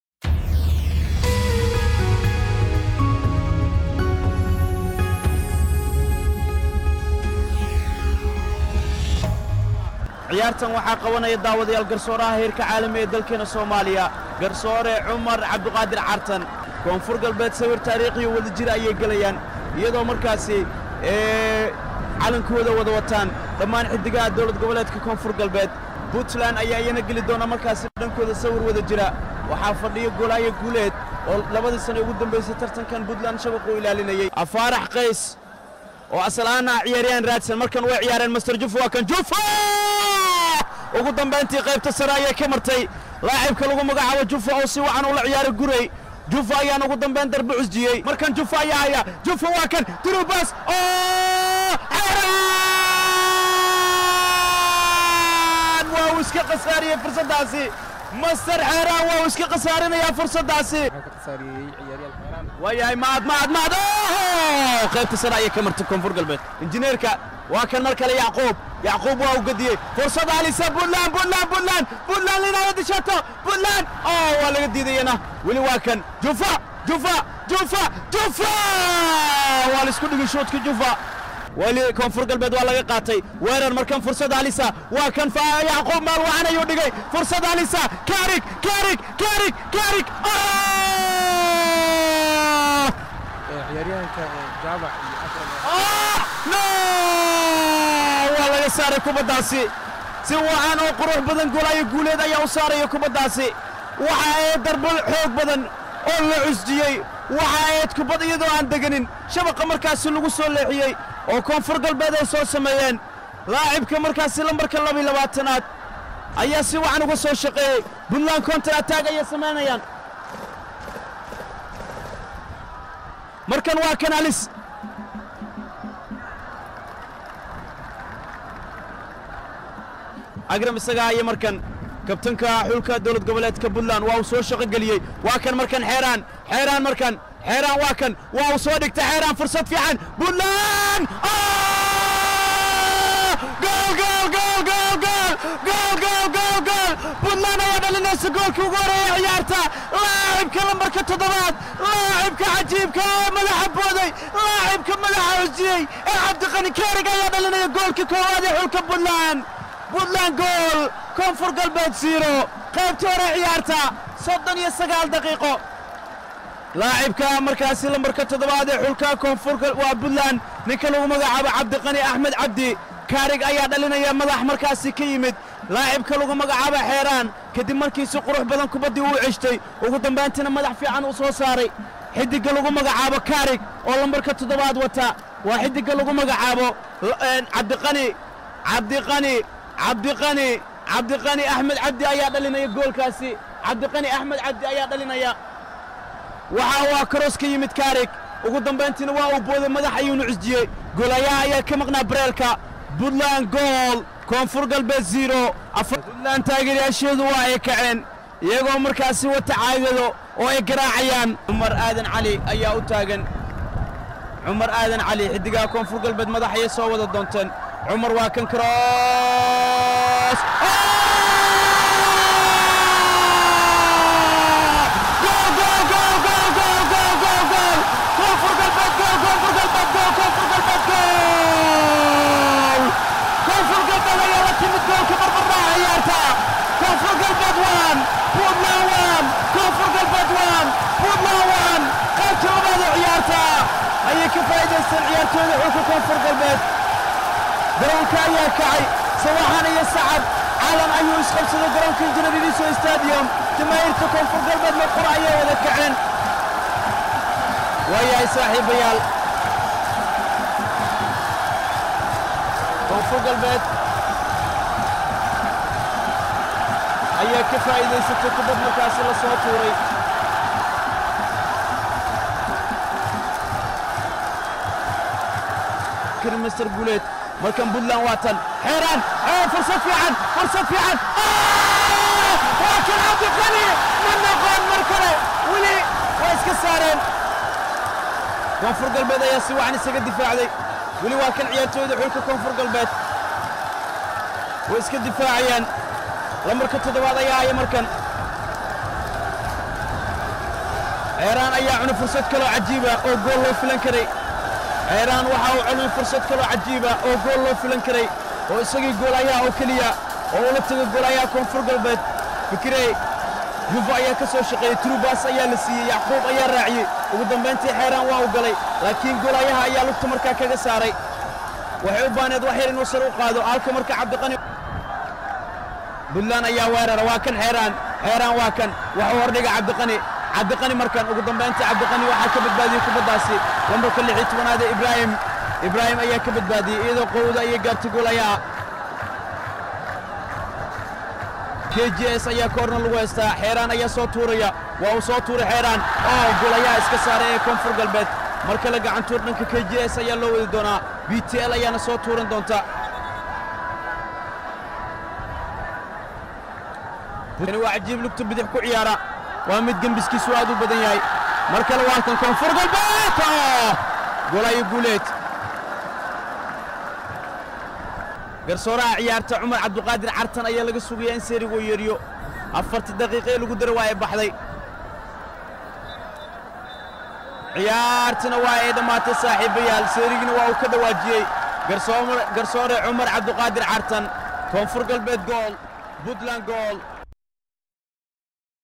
Goos goos: Goolashii Puntland iyo Koonfur Galbeed
Goos goos: Goolashii Puntland iyo Koonfur Galbeed (1-1),  Tartanka kubada cagta ee koobka Dowlad Gobaleedyada iyo Gobalka Banaadir 2021.